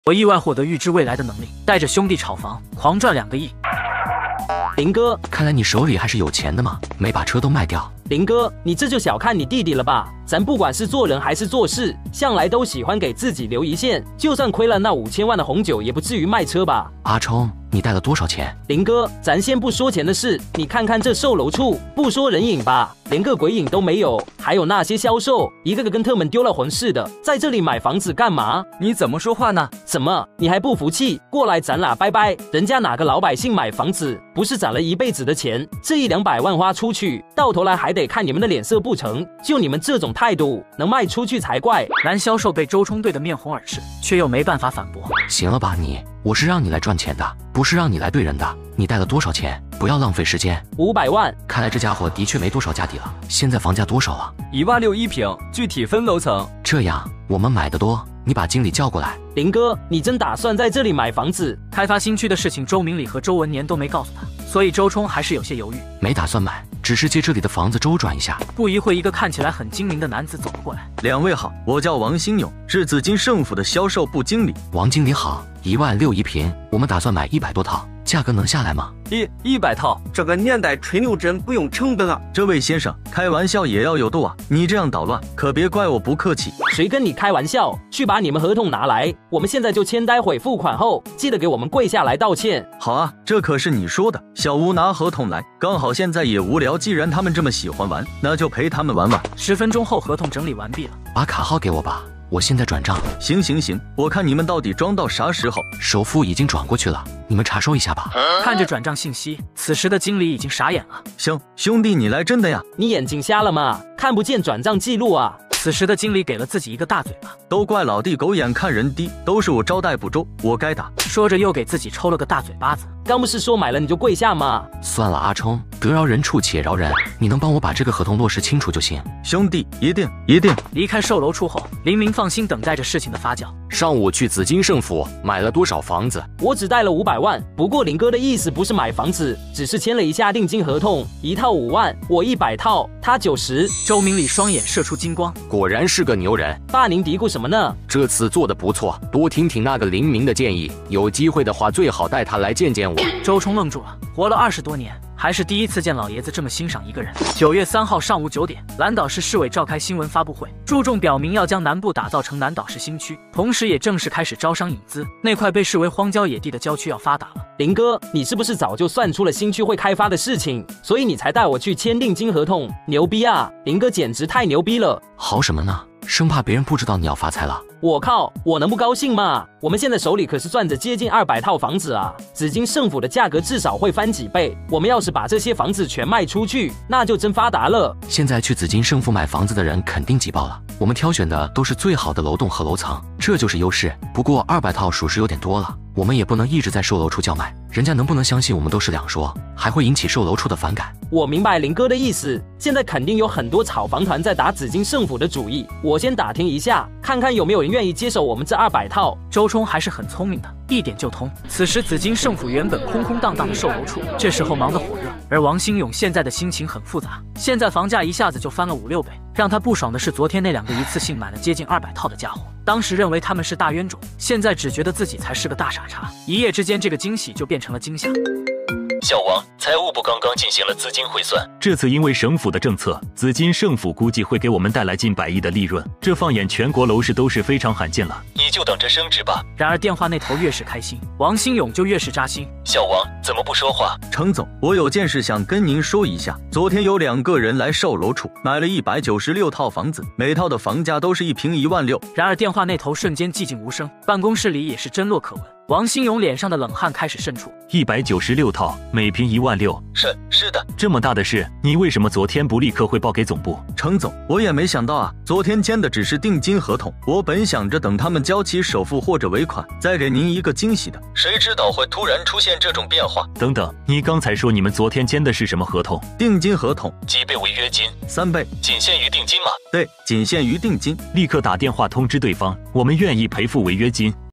意外獲得預知未來能力，帶領兄弟炒房，賺的盆滿缽滿【預測未來，我要完美人生】
用心做好每一帧动画的宝藏博主！
這裏是久遇九七
用心做好每壹幀動畫的寶藏博主
不拖更，不斷更
按時吃飯，按時睡覺。